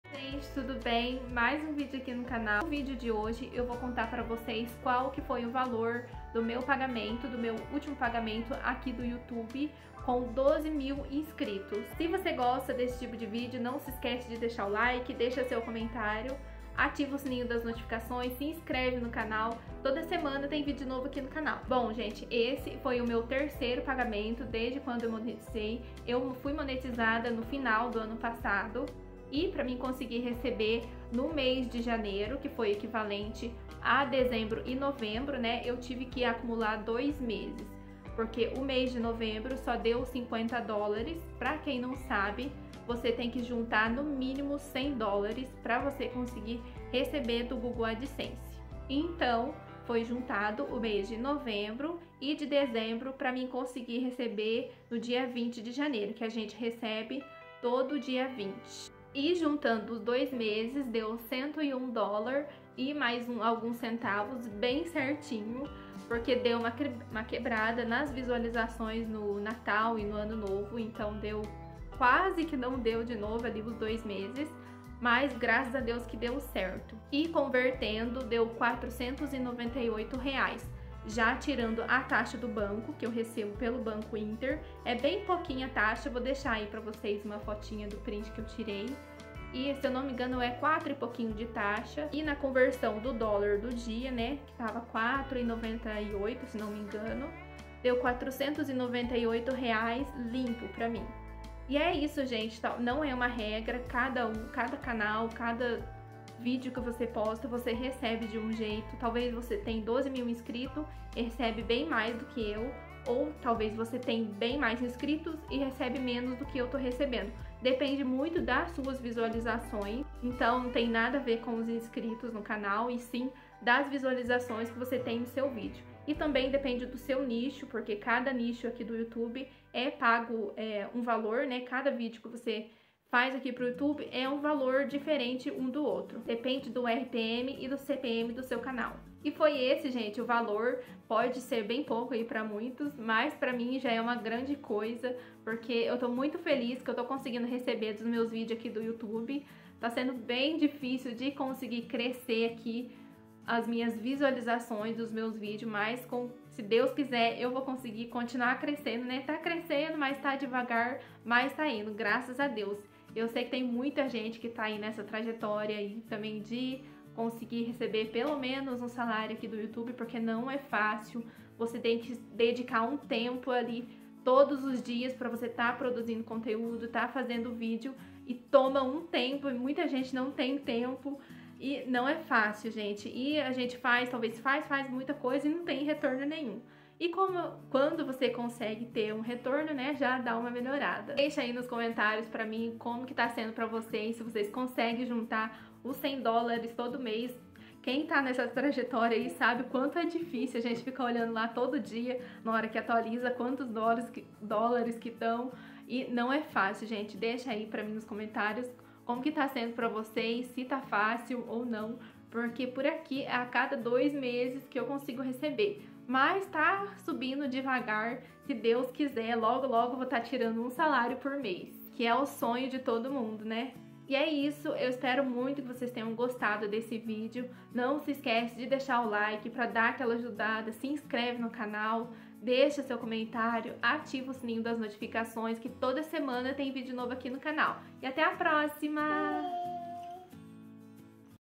Quanto recebi do YouTube esse mês com 12mil inscritos
Oi gente tudo bem esse vídeo vou contar para você o valor do meu pagamento aqui do YouTube nesse mês de Janeiro